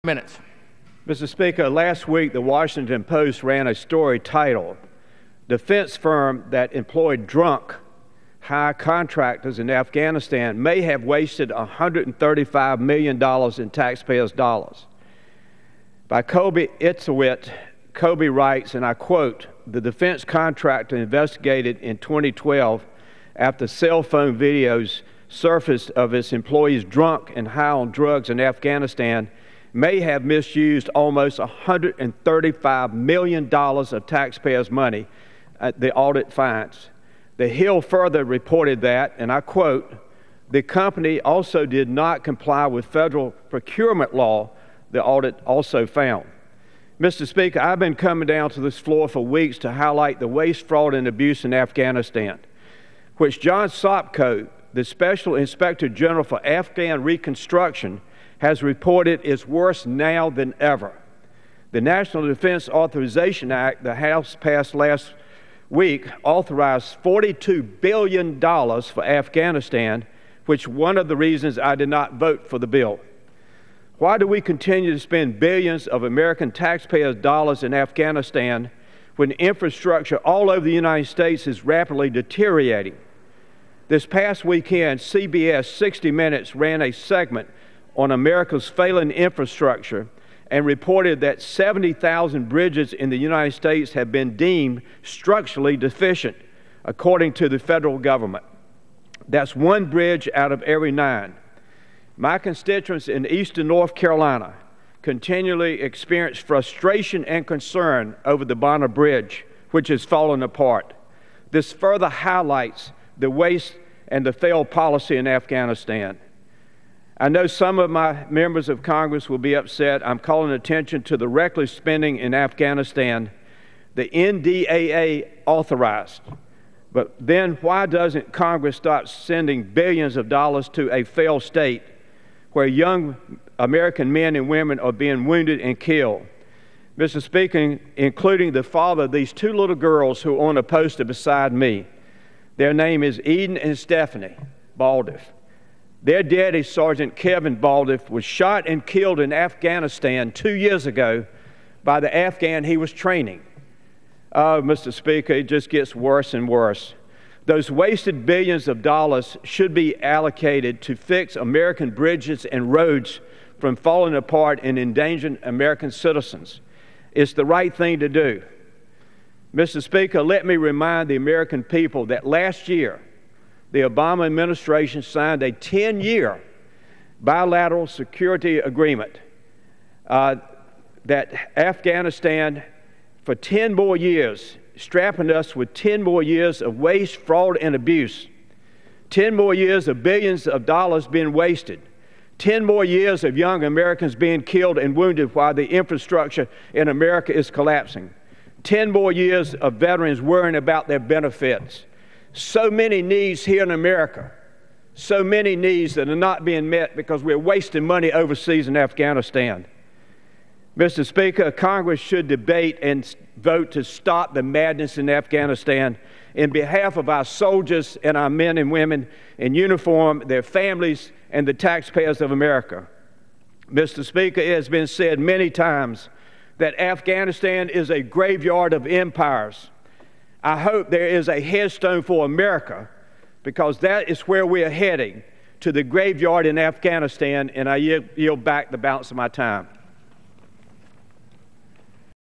May 19, 2015 Congressman Jones Raises Hell Over Defense Contractor Waste
Congressman Jones speaks on the House floor about The Washington Post's report that a defense contractor wasted $135 million in Afghanistan.